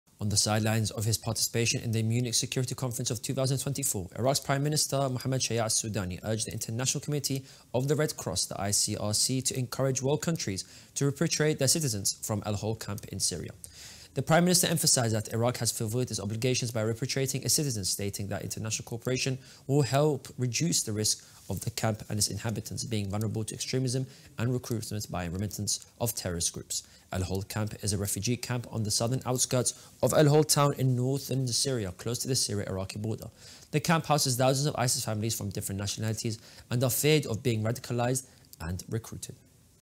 Iraqi PM urges Red Cross Committee encourage countries repatriate their citizens from al-Hol camp
Iraqi PM urges ICRC to facilitate repatriation of citizens from al-Hol camp, highlighting risk of extremism. Camp near Syria-Iraq border houses ISIS families, raising concerns of radicalization and recruitment.

On the sidelines of his participation in the Munich Security Conference 2024, Iraq's Prime Minister Mohammad Shiya Al-Sudani urged the International Committee of the Red Cross (ICRC) to encourage world countries to repatriate their citizens from al-Hol camp in Syria.
The PM emphasized that Iraq has fulfilled its obligations by repatriating its citizens, stating that international cooperation will help reduce the risk of the camp and its inhabitants being vulnerable to extremism and recruitment by remnants of terrorist groups.
Al-Hol camp is a refugee camp on the southern outskirts of al-Hawl town in northern Syria, close to the Syria-Iraq border. The camp houses thousands of ISIS families from different nationalities and are feared of being radicalized and recruited.